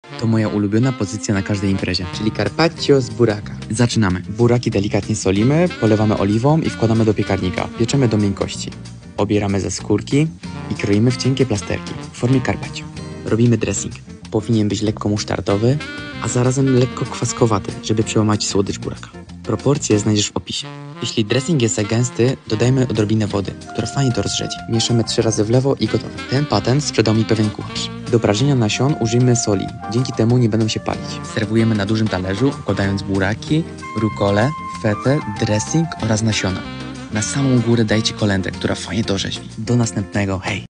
To moja ulubiona pozycja na imprezie!
CARPACCIO Z BURAKA Z FETĄ, DRESSINGIEM I NASIONAMI

Robimy!

Najlepiej dzień wcześniej upiec buraki z odrobiną soli, delikatnie polane oliwą z oliwek.
Pieczemy do miękkości w 200 C przez około 1,5 h (zależy od wielkości buraków)

Dressing
• 3 łyżki oliwy
• 1 łyżka musztardy miodowej
• 1 łyżeczka musztardy francuskiej
• 1 cytryna
• 1/2 limonki
• 1/2 łyżki miodu
• 1 łyżka wody

TIP❗️Nasiona słonecznika i dyni prażymy z dużą ilością soli dzięki temu nie będą się przypalać. A to za sprawą soli którą wchłania tłuszcz z nasion .
Smacznego! 😋
Carpaccio • fit przepis • zdrowie • sałatka